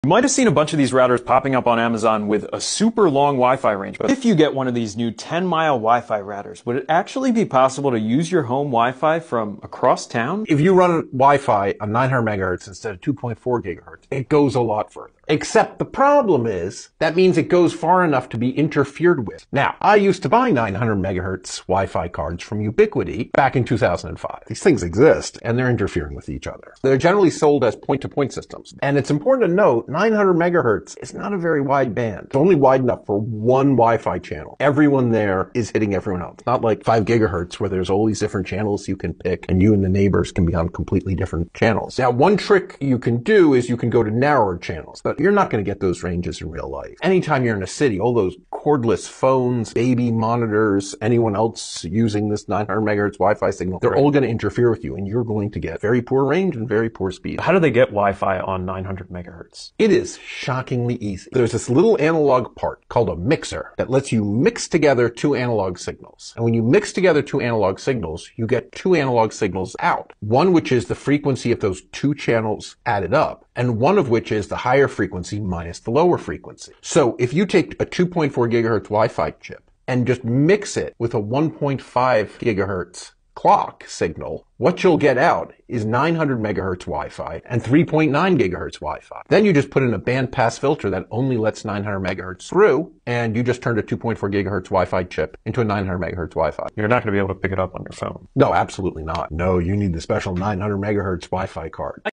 Can your WiFi router really reach 10 miles?
Can you really get Wi-Fi that reaches your iPhone from 10 miles away? Morse Micro revealed a new Wi-Fi HaLow router using 900 MHz long-range Wi-Fi—boasting up to 9.9 miles of range. But is this a game-changer for home internet, or just clever marketing?

In this video, we explain how 900MHz Wi-Fi bridges work, how it differs from Wi-Fi 6 and Wi-Fi 7, and why it’s not quite ready to replace your home router. While the range sounds amazing, low bandwidth, signal interference, and extra hardware needs make this tech better for IoT, smart farming, and large outdoor properties than for home networking or mobile devices. Your iPhone or Macbook won’t be able to pick up the 900MHz signal (without a dongle). You can already find similar 900 MHz devices marketed as Wi-Fi Bridges on Amazon—many claim up to 5 km of wireless range. We’ll break down what you’re really getting and whether it’s worth it.